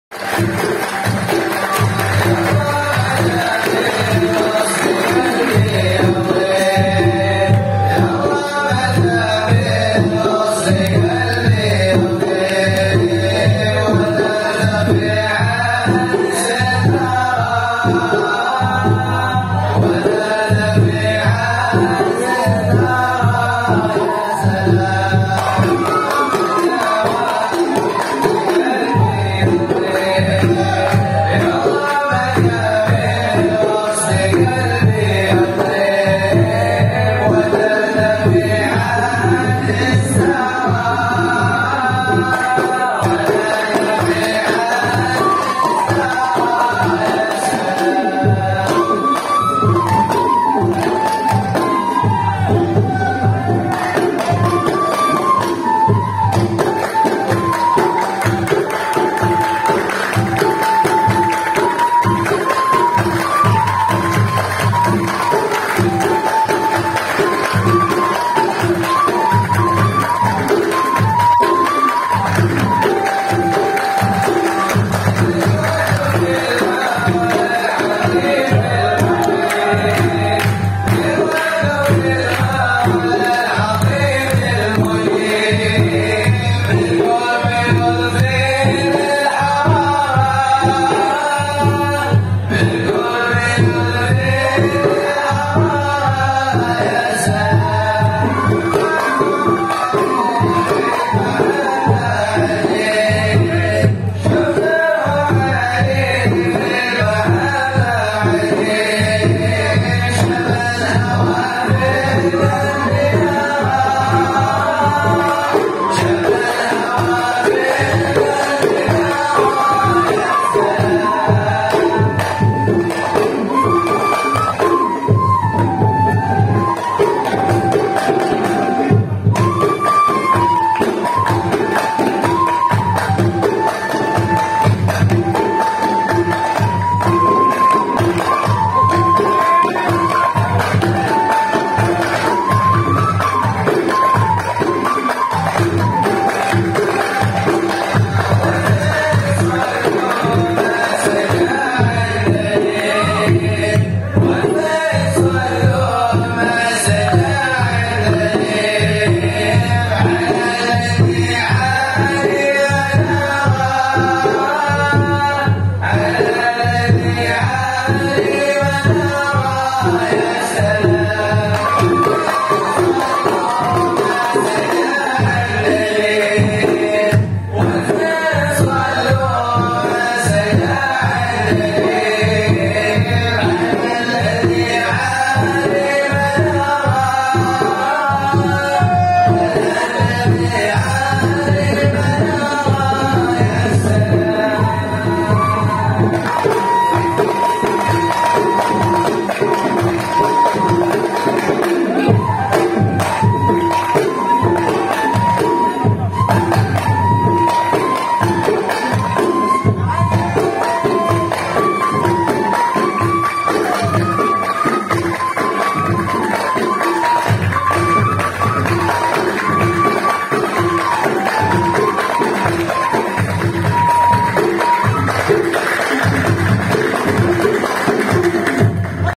Hajir Marawis dihadiri Habaib Hadromaut PP Sunniyah Salafiyah Jawa Timur
Hajir Marawis dihadiri Habaib Hadromaut PP Sunniyah Salafiyah Jawa timur diantara nya Putra Habib AbdulQodir bin Ahmad Jeddah